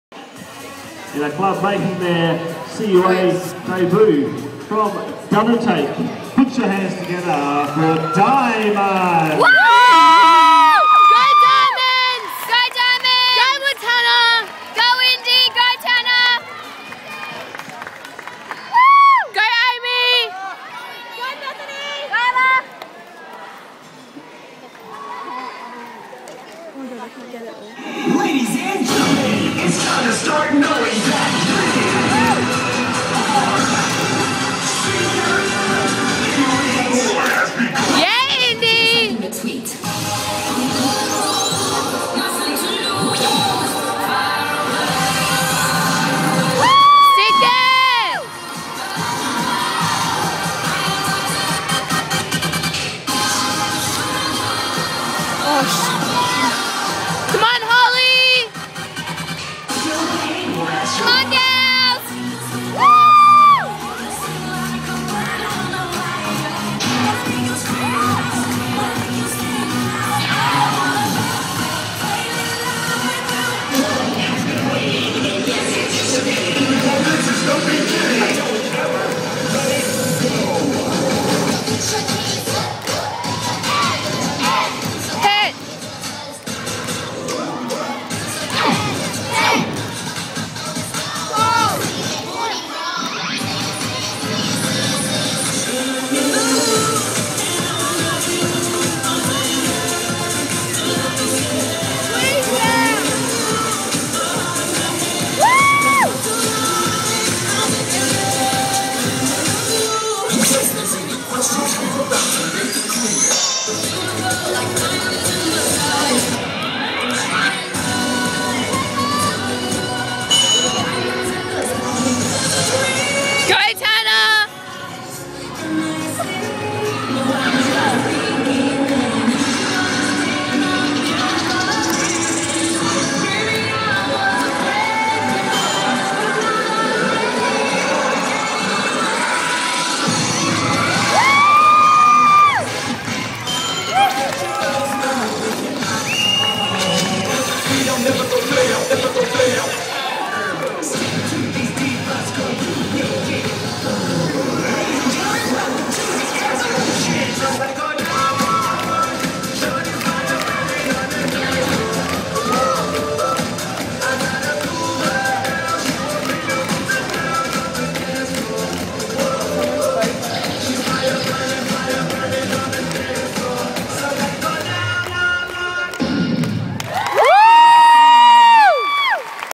Double Take Diamonds Senior Elite 2
the double take diamonds on their debut competition- placing 2nd in the Senior Level 2 division